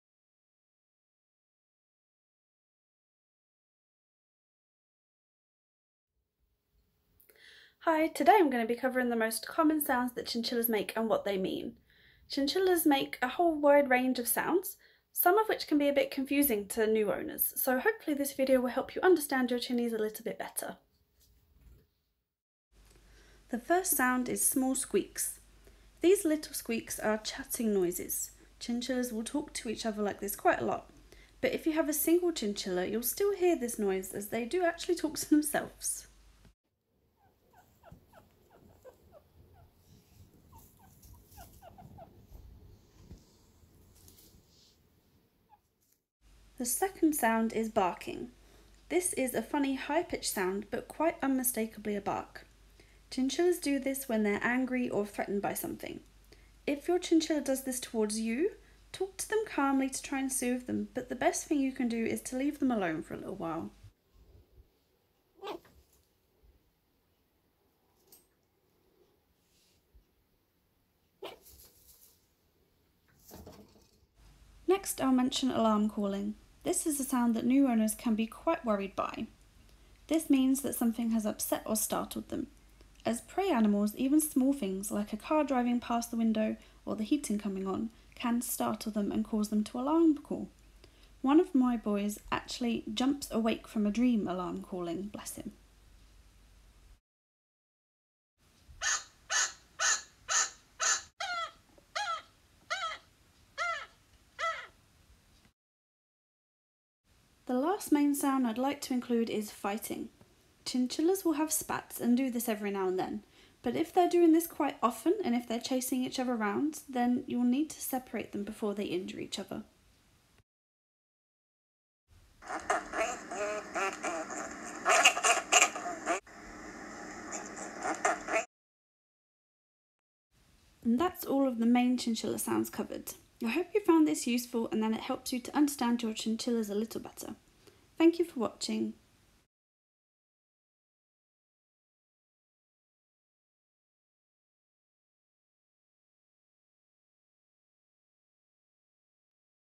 Common Chinchilla Sounds
**Join our friendly Facebook Group “Chinchillas UK” for advice and in-depth guides on looking after your chinchillas!**

In this video I have covered all of the most common chinchilla sounds, and what they mean. I haven't done every single sound, just the ones you will hear most often.
Small squeaks - 0:37
Barking - 1:04
Alarm calling - 1:39
Fighting - 2:02